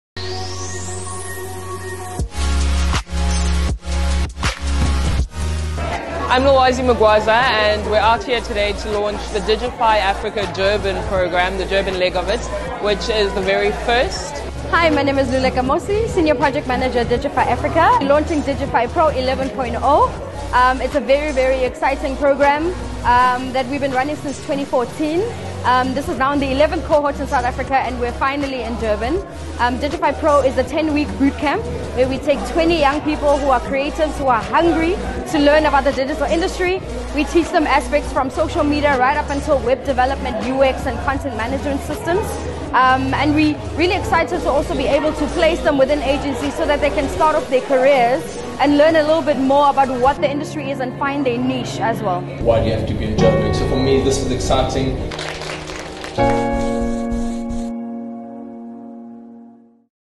Digify PRO goes live in Durban
Watch the launch of flagship programme, Digify PRO, in Durban